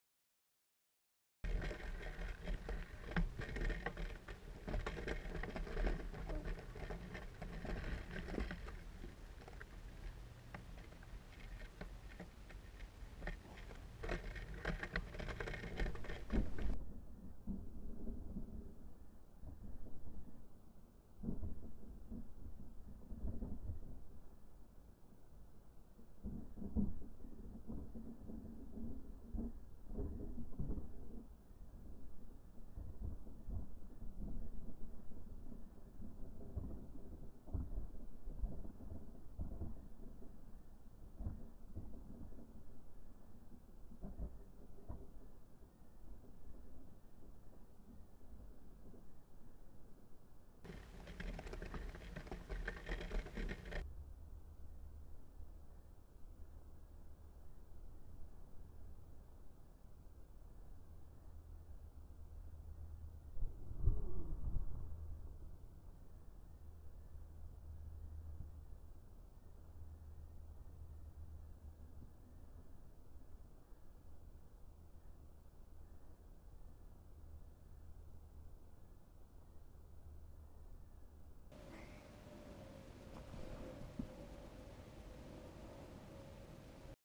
720p& WVGA Go Pro 3 SlowMo Bike & Bouncy Castle
Go Pro 3 720p 120fps (not slowed) and WVGA 240fps testing
bear in mind that WVGA is an SD resolution (it shoots at 640 x 480),